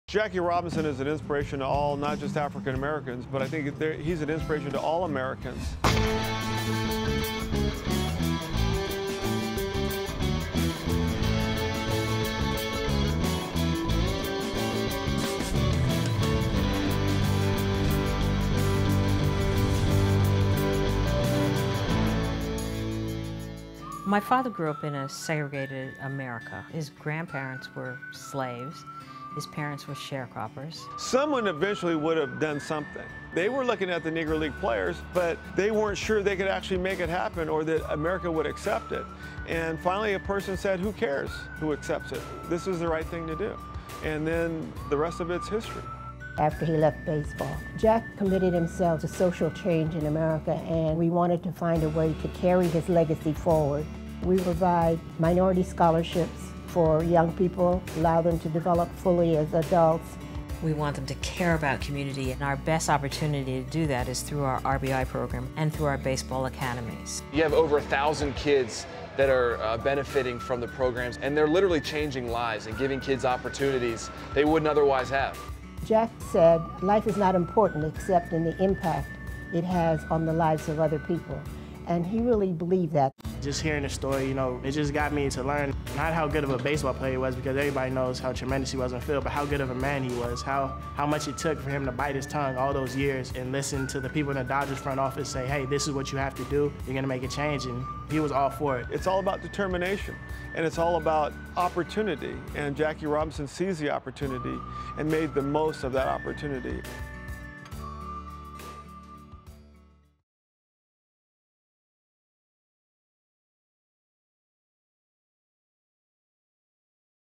Sports in America, Waiting for Jackie (Portuguese Subtitles)
Part of the "Sports in America Series," this video shares the story of the Jackie Robinson Foundation. Jackie Robinson was the first African American to play Major League Baseball. He once said that, "life is not important except in the impact it has on the lives of other people" and after his baseball career ended, he dedicated himself to social change in America. To continue his legacy, his family created the Jackie Robinson foundation, which creates minority scholarships for young people, the RBI program for kids, and baseball academies. (Portuguese Subtitles)